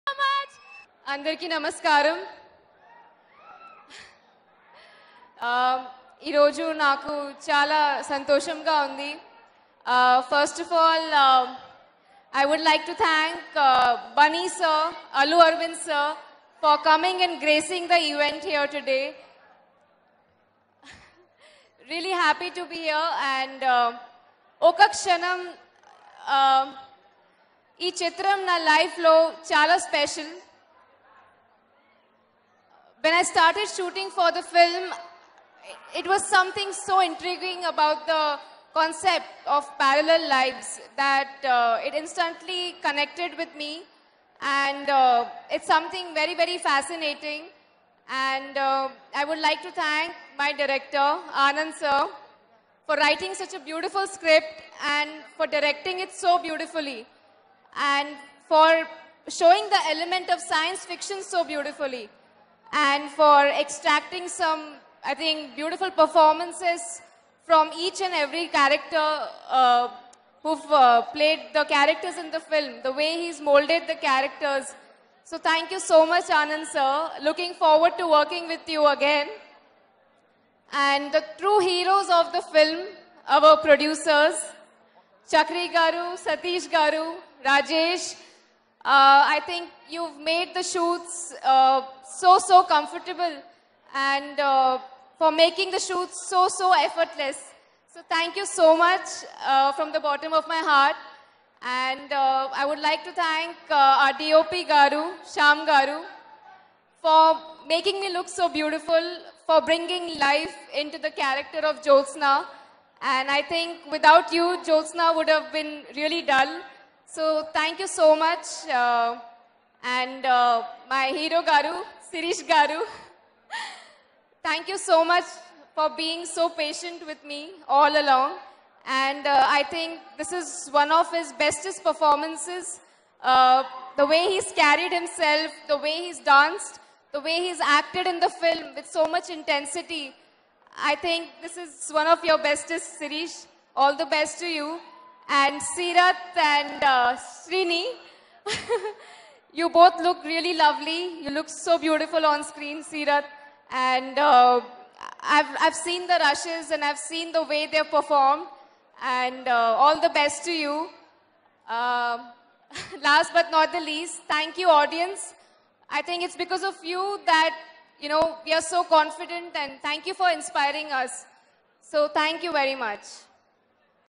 Surbhi Superb Speech @ Okka Kshanam Movie Pre Release Event || Movie Stop
Watch Surbhi Superb Speech @ Okka Kshanam Movie Pre Release Event || Movie Stop